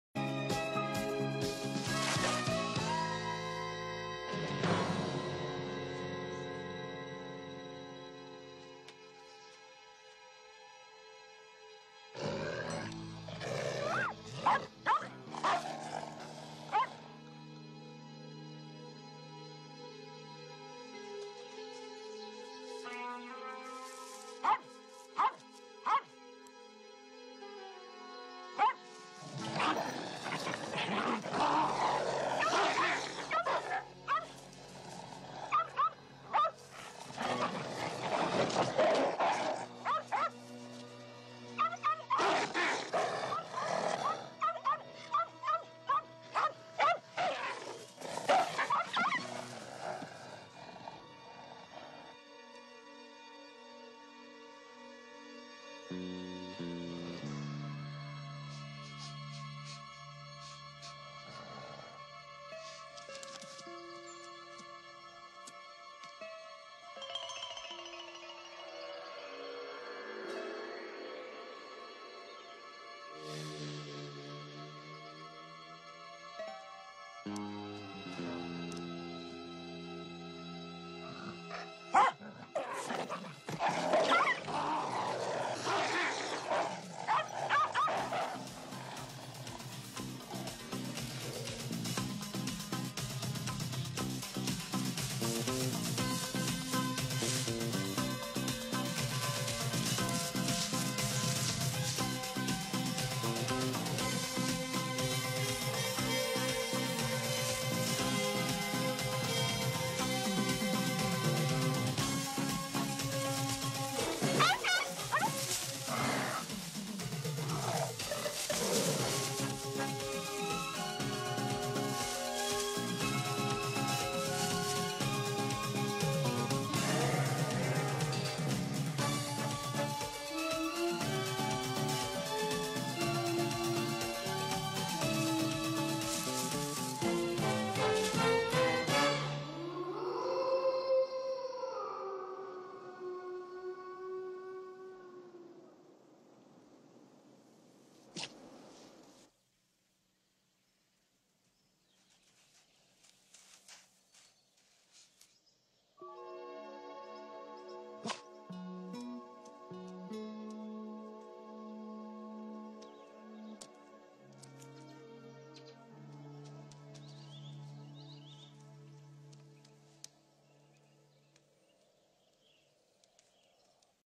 hungamanews
National Geography channel paid 1 million dollar to the person who shot this video, where a small dog is saving three tiger cubs from a wolf.